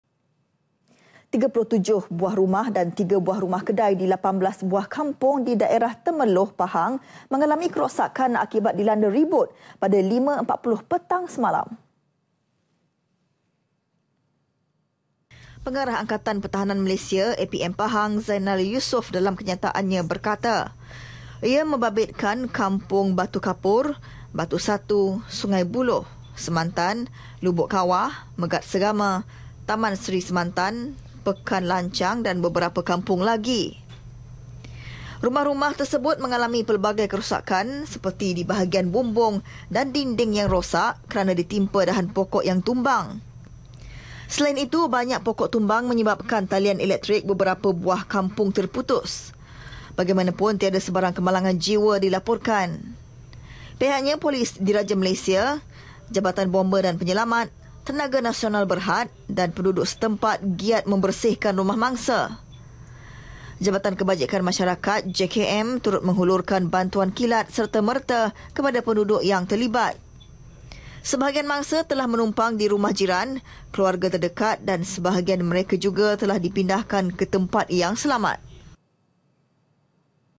RIBUT DI TEMERLOH – 37 RUMAH DAN 3 KEDAI DI 18 BUAH KG ALAMI KEROSAKAN [11 OKT 2016]
37 buah rumah dan 3 buah rumah kedai di  18 buah kampung di daerah Temerloh, Pahang mengalami kerosakan akibat dilanda ribut, pada 5.40 petang semalam .